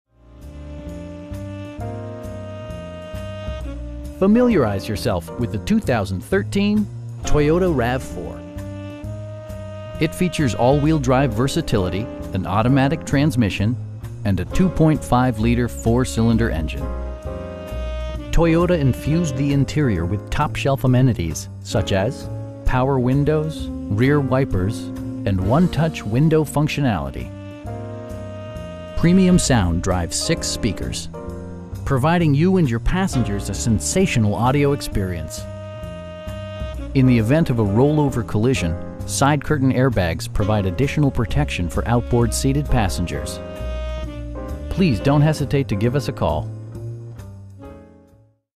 2013 Toyota RAV4 Limited 4WD in Vienna, VA 22182
Familiarize yourself with the 2013 Toyota RAV4. It features all-wheel drive versatility, an automatic transmission, and a 2.5 liter 4 cylinder engine. Toyota infused the interior with top shelf amenities, such as: front bucket seats, air conditioning, power door mirrors, power windows, an overhead console, rear wipers, and 1-touch window functionality. Premium sound drives 6 speakers, providing you and your passengers a sensational audio experience. In the event of a rollover collision, side curtain airbags provide additional protection for outboard seated passengers. We pride ourselves in the quality that we offer on all of our vehicles. Please don't hesitate to give us a call.